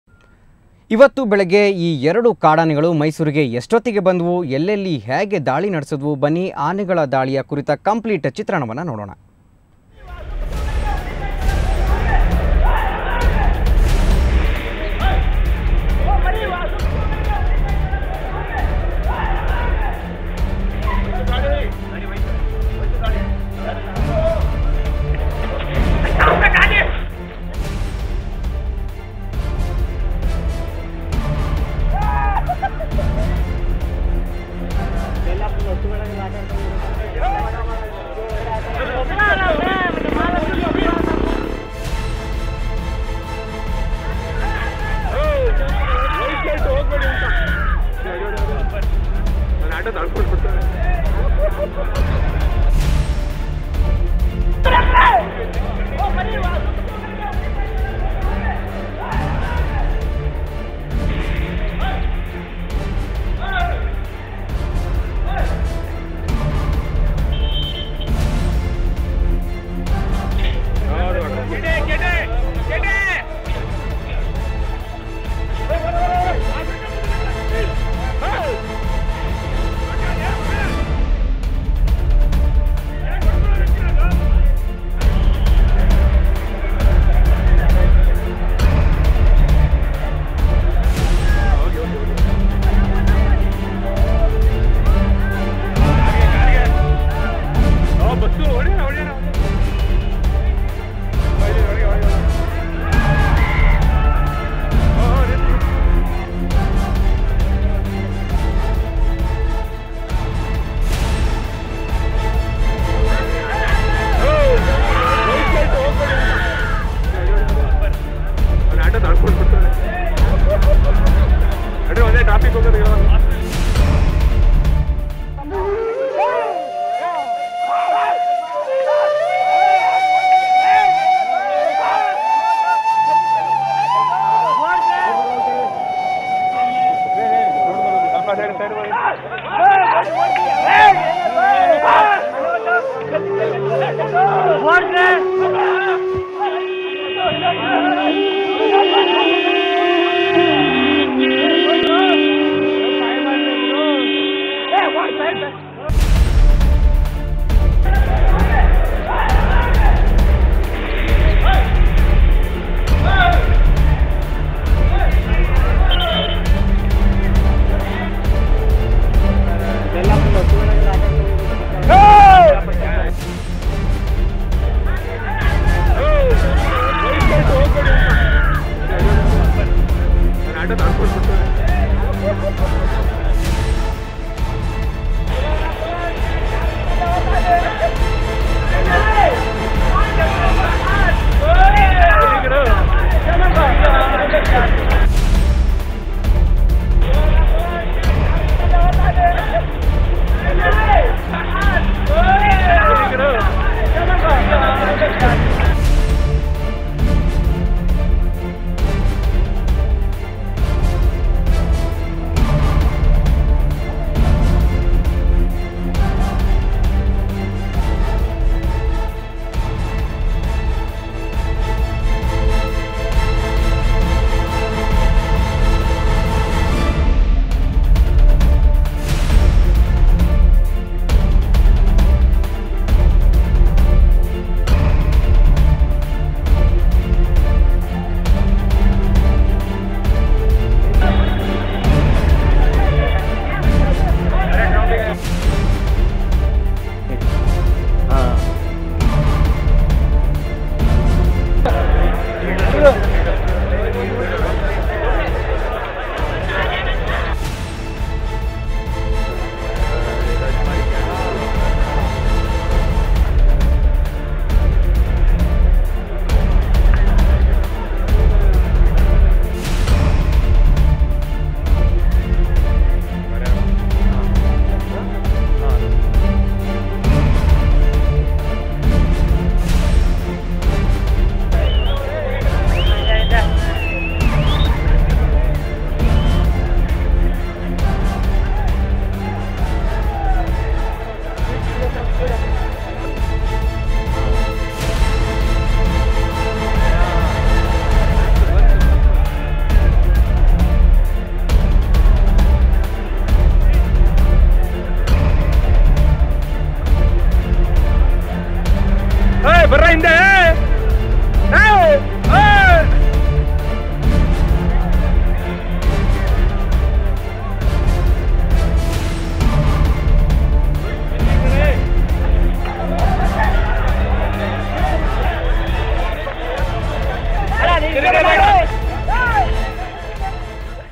SUVARNA NEWS - ELEPHANT ATTACK - COMPLETE REPORT
Here is the complete report of this elephants attack in Mysore.. Terror of Elephants in Mysore has been in control... 4 elephants had entered Mysore city and killed 2 persons and 2 cows with an unusual aggressive behaviour... Tranquilizers were used against these killer elephants... Officials successfully caught one elephant... They are trying to catch other killer elephants... Elephants, that belong to Mysore palace, particularly Abhimanyu, are used in this operation... were also used to control these wild animals... Karnataka Chief Minister announced the compensation for the loss in this elephants attack... He also promised to put a trench across the line that divides Forest and City...